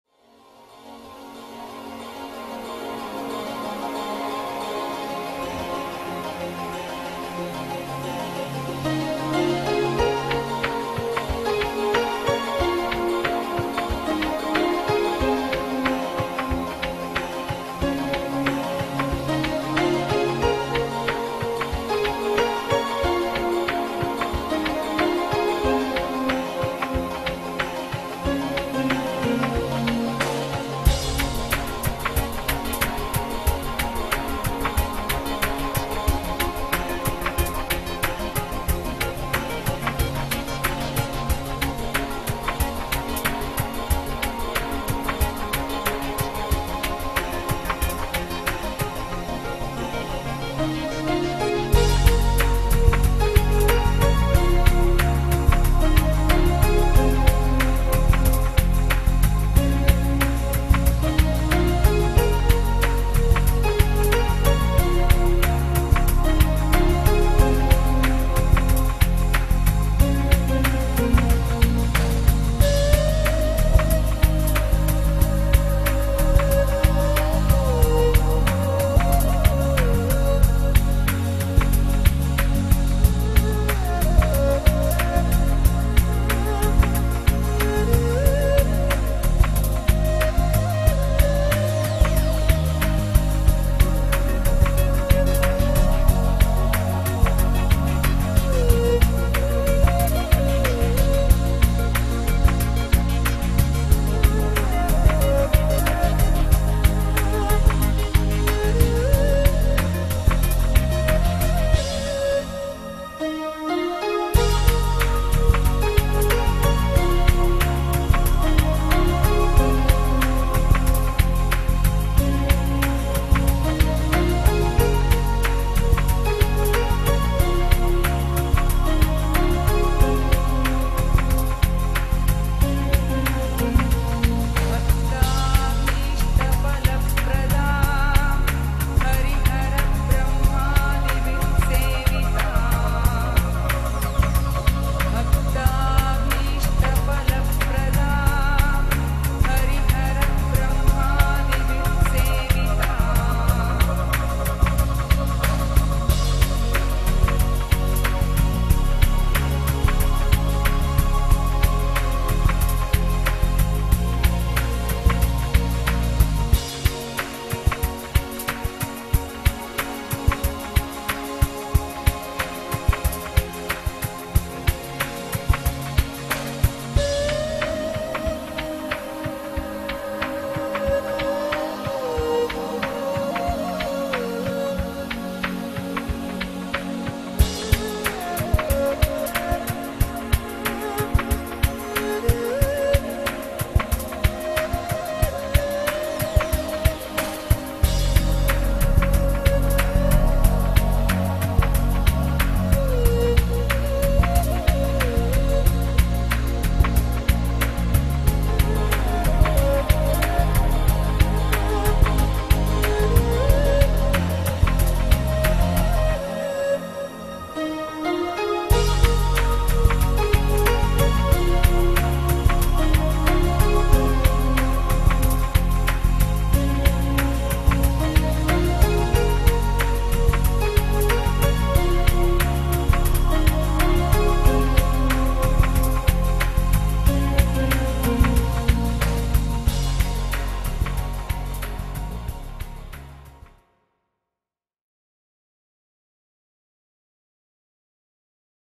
Iara's Underwater Model Demo Reel by Fathom Films
Model: Iara Mandyn

2010 underwater demo reel - swimming, posing, natural buoyancy control, self-supported mask-less regulator breathing, inverted underwater posing